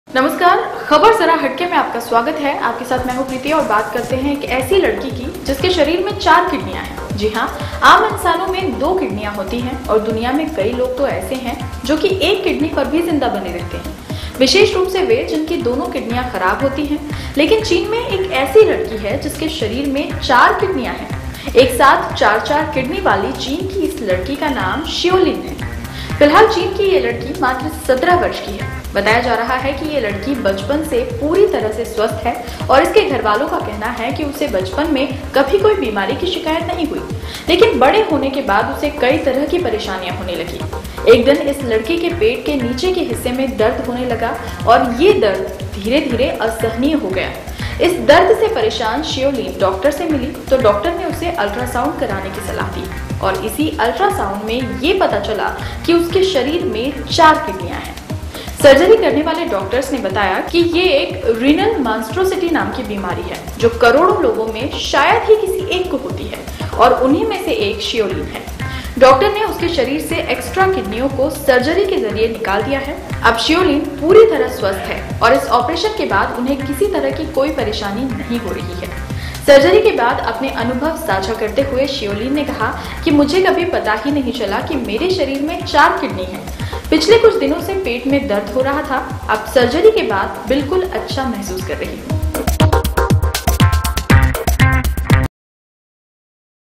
एक लड़की के शरीर में चार किडनियां
बीजिंग। आम इंसानों में दो किडनियां होती हैं और दुनिया में कई लोग तो ऐसे हैं जो कि एक किडनी पर भी जिंदा बने रहते हैं। विशेष रूप से वे जिनकी दोनों किडनियां खराब होती हैं, लेकिन चीन में एक ऐसी लड़की है जिसके शरीर में चार किडनियां हैं। एक साथ चार-चार किडनी वाली चीन की इस लड़की का नाम शियोलिन है।